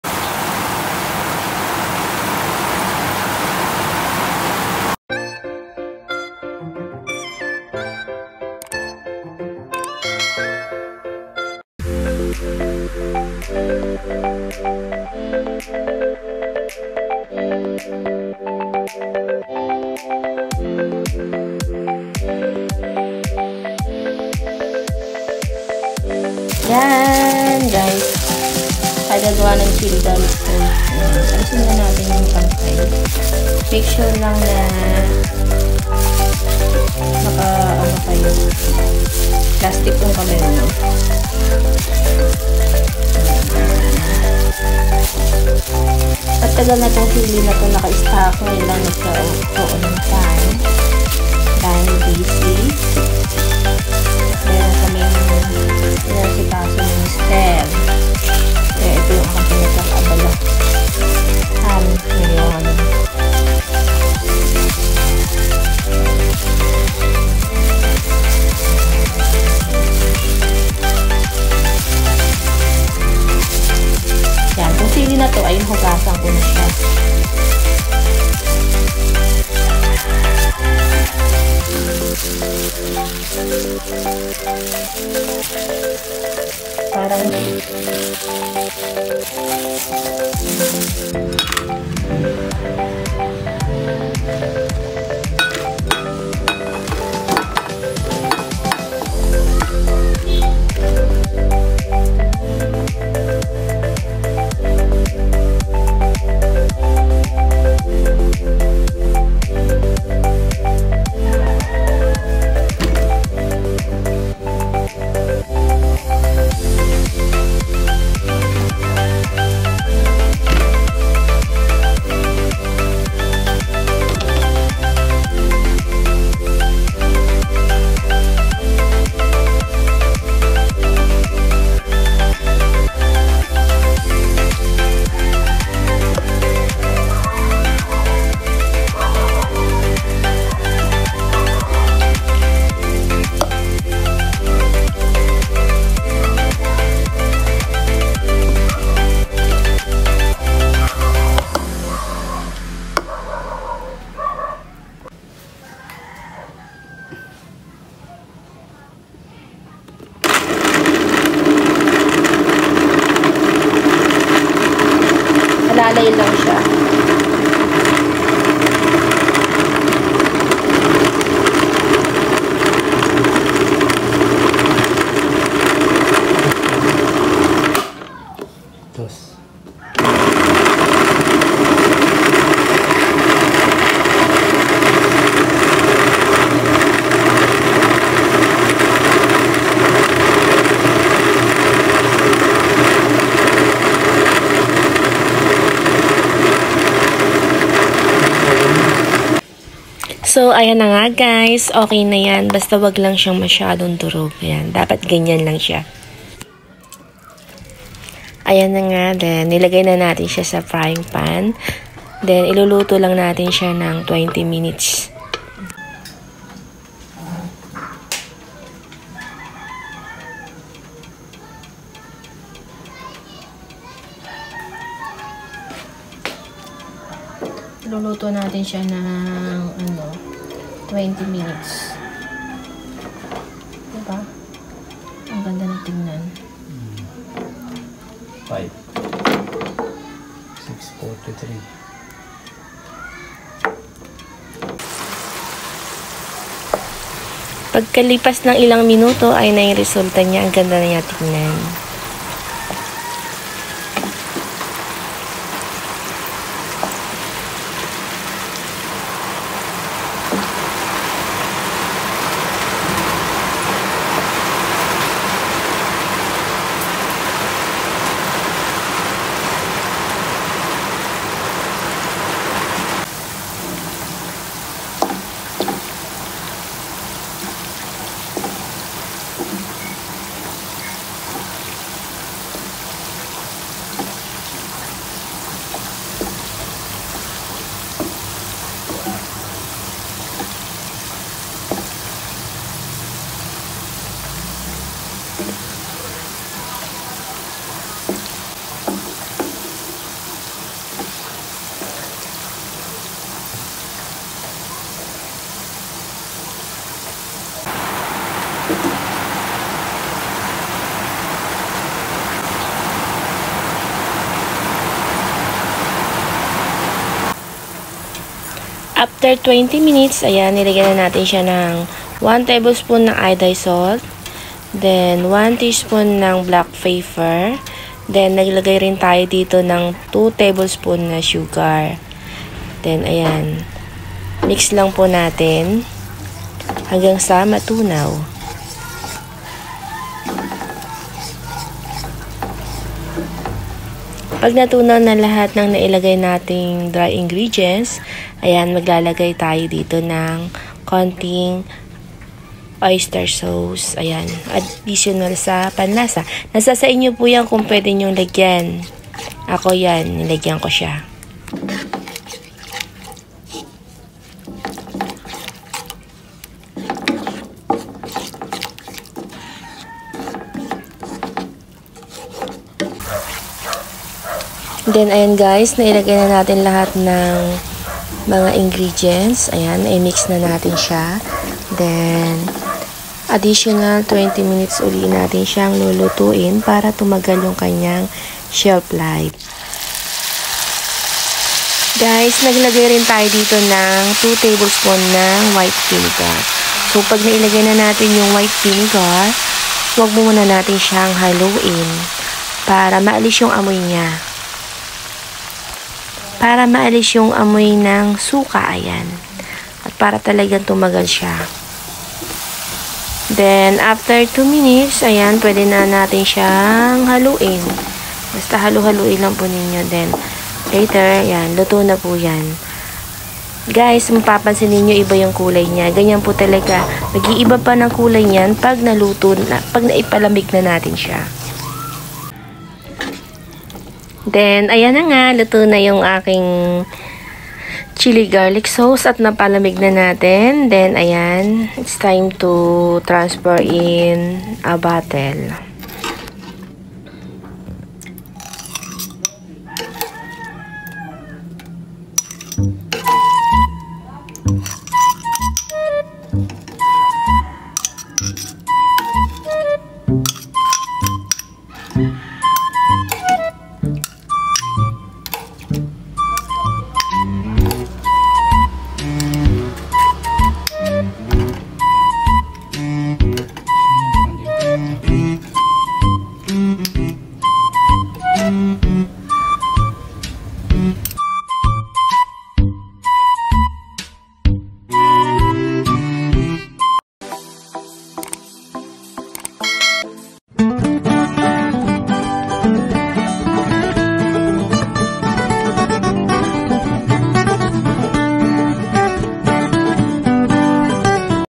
Chili Garlic Sauce Long Term Stock
1/4 red chili 32grams (turo)
4 pcs whole garlic
3 cups oil
1 tbs salt (iodized salt)
1 tsp black pepper
2 tbs sugar
1/4 tsp oyster sauce
2 tbs white sugar

Thanks everyone for watching my videos. Keep safe everyone. Godbless you all!